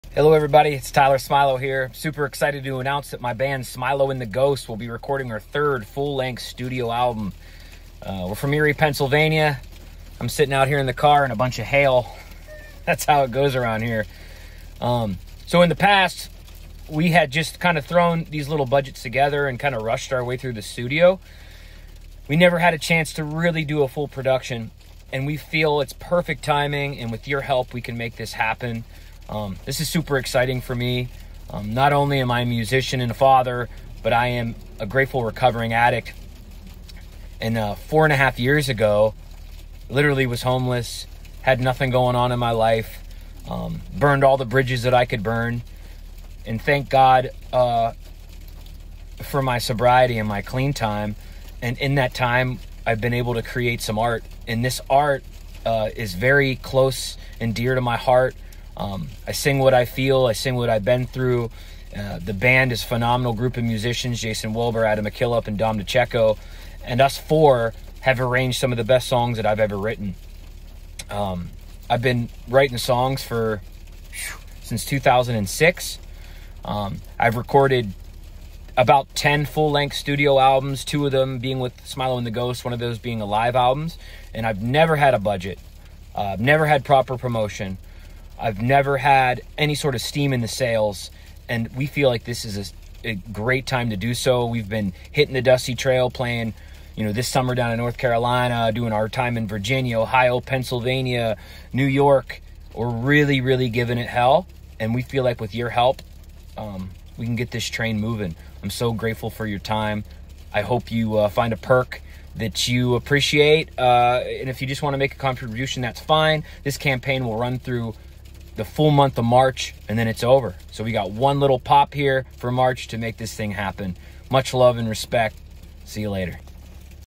Inspiring the Rust Belt with songs of Hope & Hurt
Our Indiegogo campaign video. The Campaign runs for 30 days beginning 3/1/23. With your help we can really knock this new album out of the park.